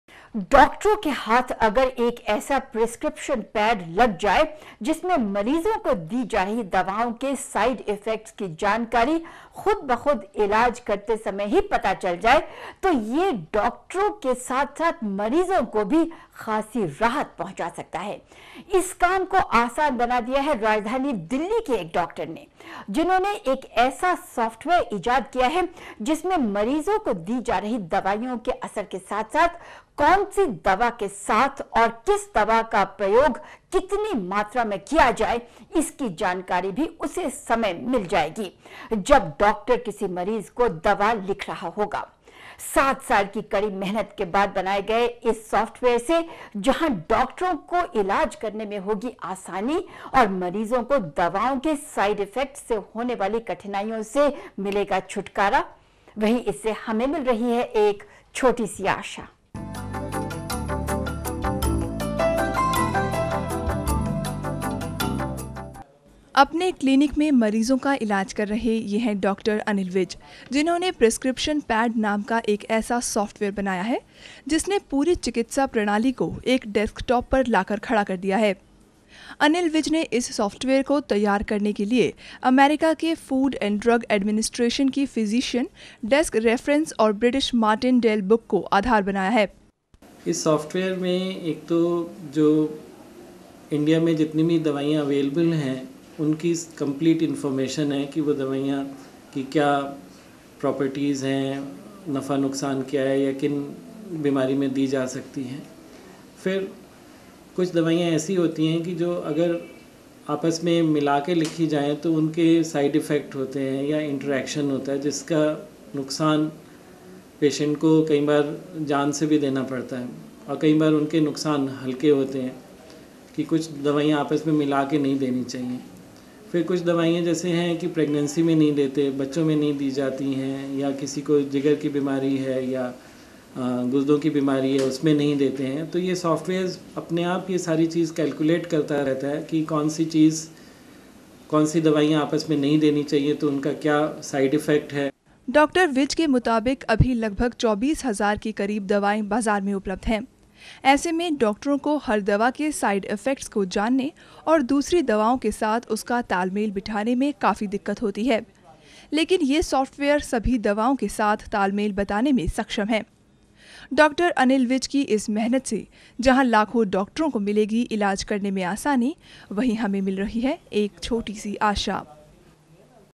Prescription pad
Ability to search the ideal drug for a patient through advanced search options.

Complete vaccination schedule for children with automatic reminder letter generation facility.

Features to calculate cardiac risk factor , obesity & body mass index, expected date of delivery Ability to keep record of every visits of individual patient in chronological order.

Facility to see pt's various investigations in bar chart.

Facility to take print-out of various dietary advices, exercises, dos & don't hand outs for patient education.